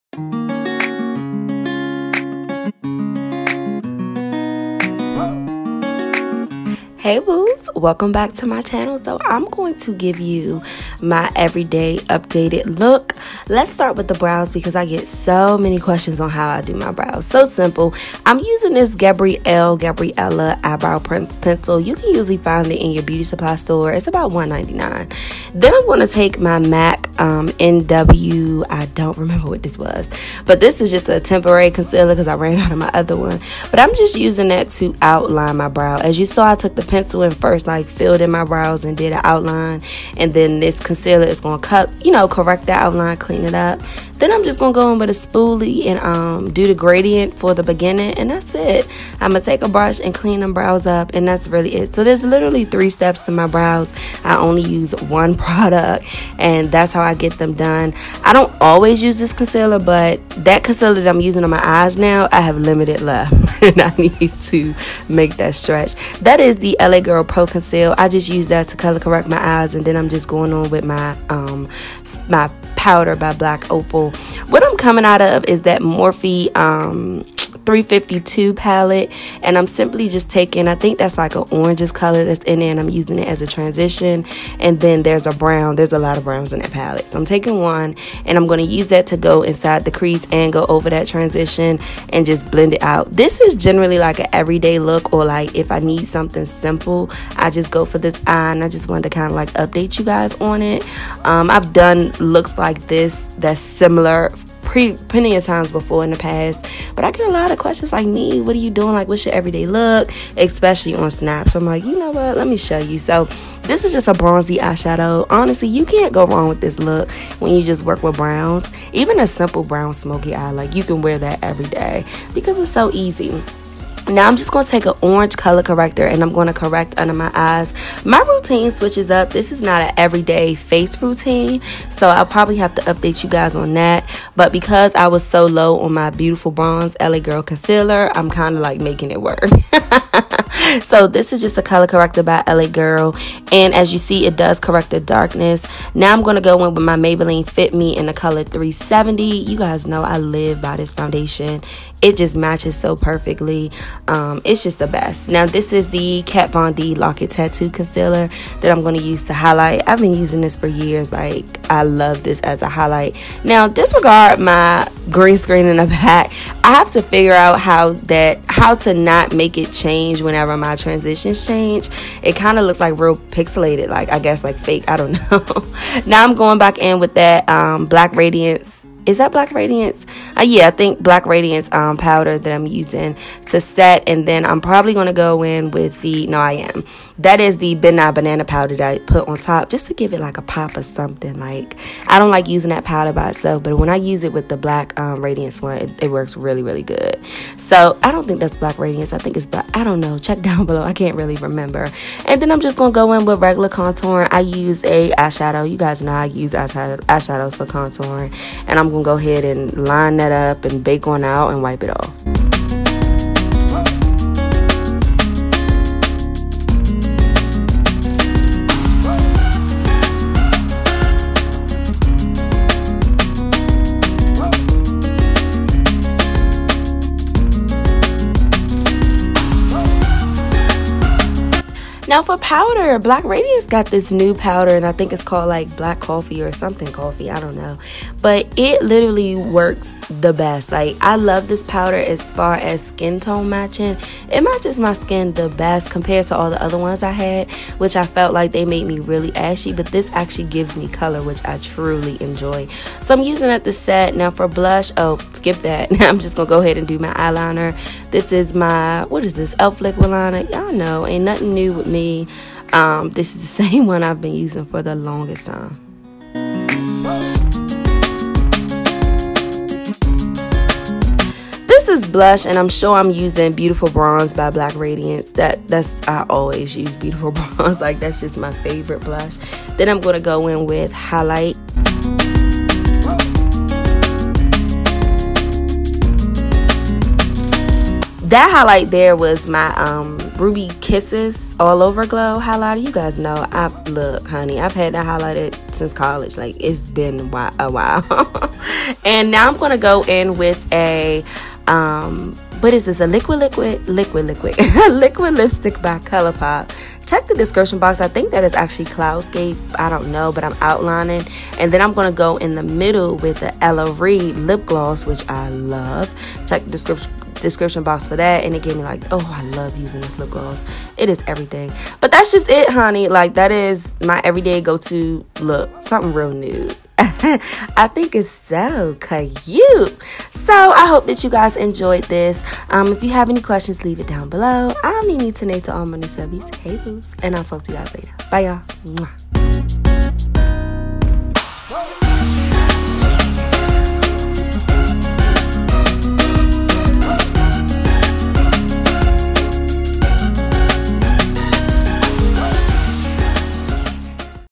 My Go To Easy Bronze Eye Makeup Tutorial
Hey Boo's! Yalll been asking on snap how I've been slaying lately so I wanted to show you all my go-to easy eye look. I hope you enjoy

Eyes: Morphe 350 2 Palette
Lashes: Ardel Wispies
Foundation: Maybelline FitMe Foundation 370
Concealer: LA Pro Conceal "Orange" color corrector" + Kat Von D Lock it Tattoo Concealer
Highlight (undereye): Black Opal Face Powder- Medium + Ben Nye Banana Powder
Contour:  Morphe 350 2 Palette Eyeshadow
Powder: Black Radiance Powder "Black Coffee"
Blush: Black Radiance Blush "Toasted Almond"
Highlight: Ruby Kisses All Over Glow "Bronze Glow"
Lips: Colourpop "Cloudscape" + Ellarie Lipgloss "Steelo"
Hair: girl it's a pony using some bundles

Camera Used: Canon T3i
Lens: Sigma 30mm F1.4
Editing: Windows Movie Maker + Sony Movie Studio Platinum 13